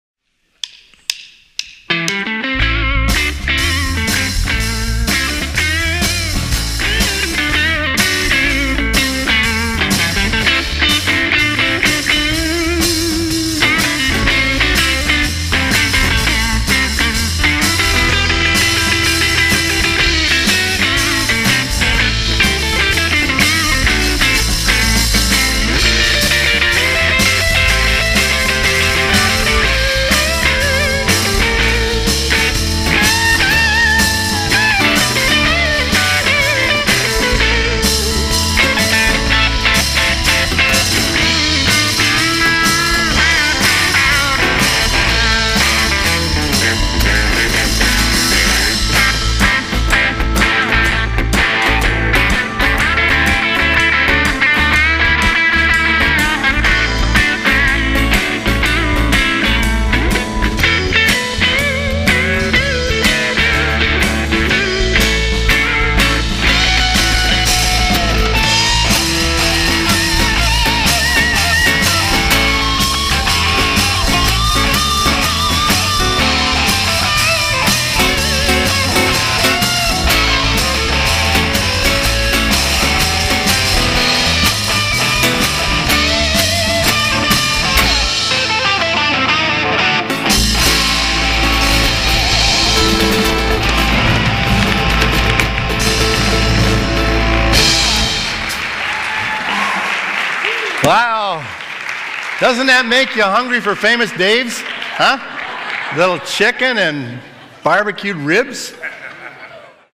Offering Blues
Playing some blues for the offering special at church